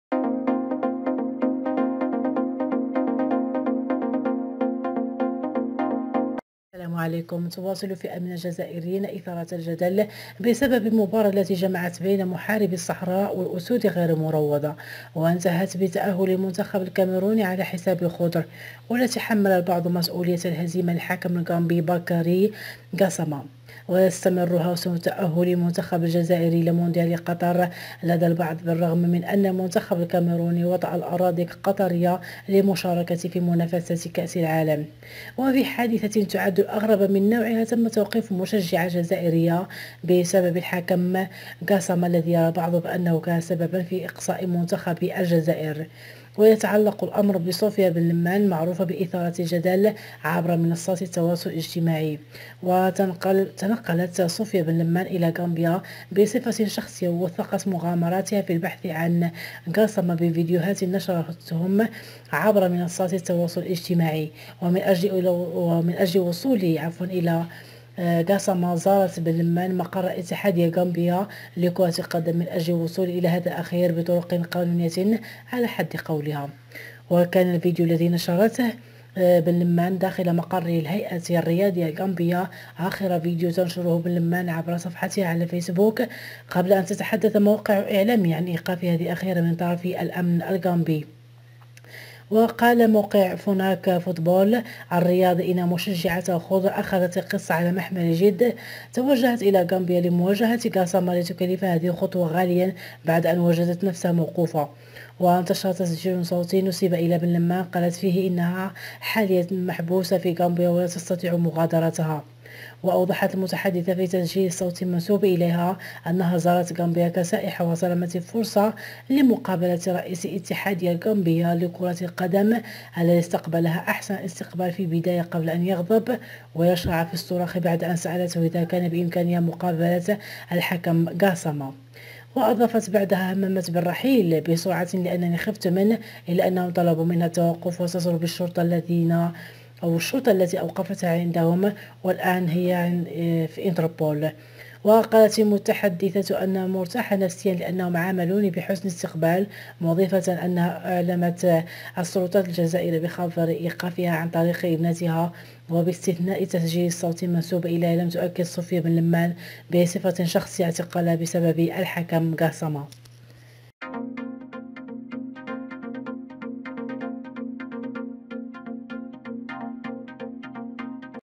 اعتقال مشجعة جزائرية ذهبت إلى منزل غاساما وطلبت مشاركة الجزائر في كأس العالم
تعرض القناة الجيدة المباشر الآن على مدار الساعة جديد الأخبار والقضايا المغرب
 والجزائر وإسبانيا وفرنسا ومصر وتونس وليبيا والعالم